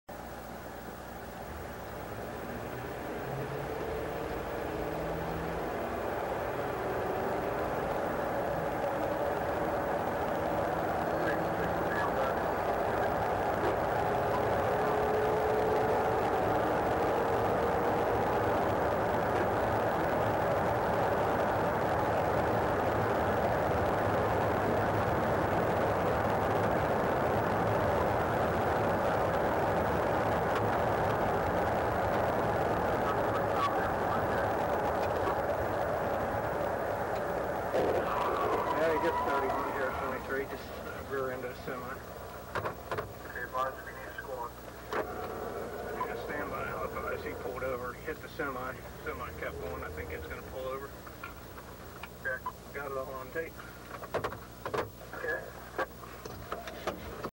Suspected Drunk Rear Ends Semi
Trooper following a driver he suspects of being intoxicated catches on his dashboard camera the driver rear ending the trailer of a semi driving directly in front of him.